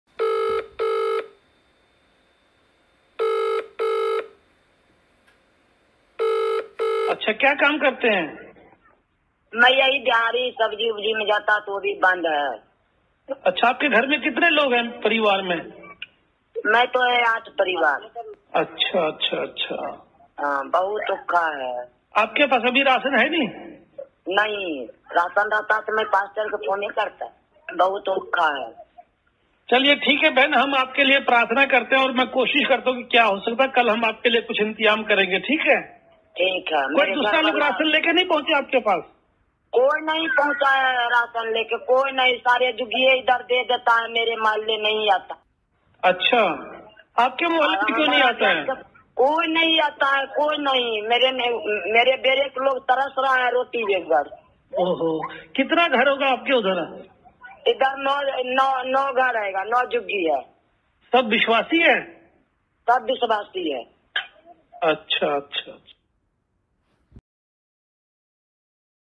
Emergenza covid-19
L'isolamento sta avendo un effetto devastante in luoghi dove alla persecuzione si aggiunge, oggi, l'emergenza pandemia.

In questo breve video potrai ascoltare uno scambio telefonico tra una donna cristiana e il suo pastore, uno dei tanti esempi di quello che i nostri partner locali in india affrontano ogni giorno.